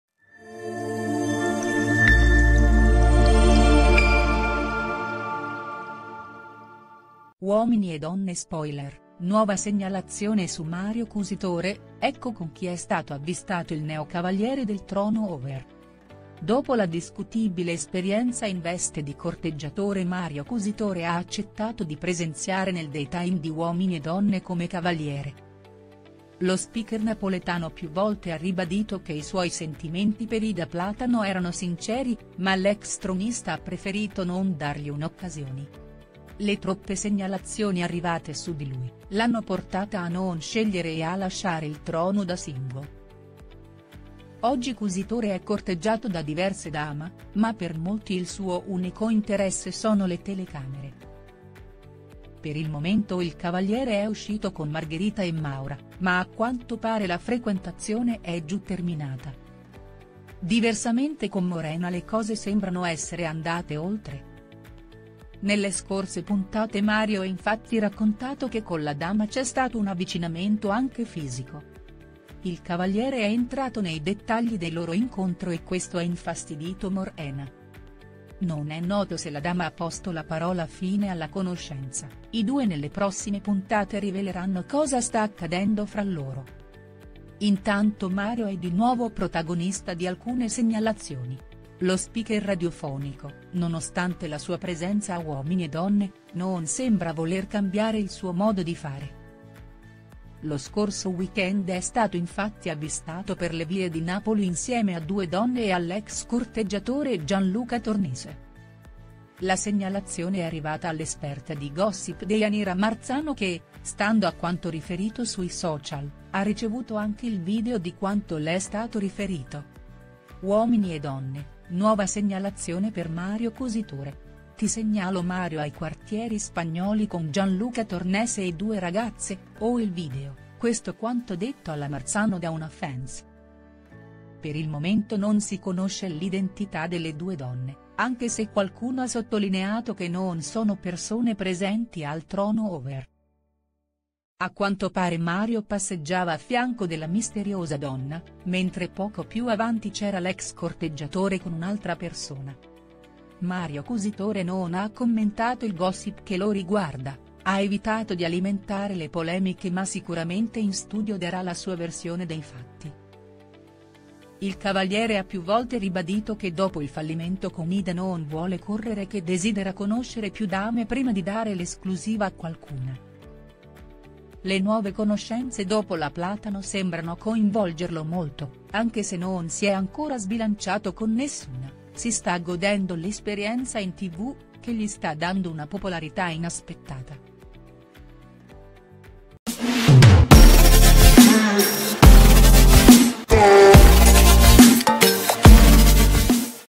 Uomini e Donne spoiler la segnalazione choc su Mario Cusitore, con chi è stato avvistato
Uomini e Donne spoiler: nuova segnalazione su Mario Cusitore, ecco con chi è stato avvistato il neo cavaliere del trono over.

Dopo la discutibile esperienza in veste di corteggiatore Mario Cusitore ha accettato di presenziare nel daytime di Uomini e Donne come cavaliere. Lo speaker napoletano più volte ha ribadito che i suoi sentimenti per Ida Platano erano sinceri, ma l’ex tronista ha preferito non dargli un occasioni. Le troppe segnalazioni arrivate su di lui, l’hanno portata a non scegliere e a lasciare il trono da single. Oggi Cusitore è corteggiato da diverse dama, ma per molti il suo unico interesse sono le telecamere.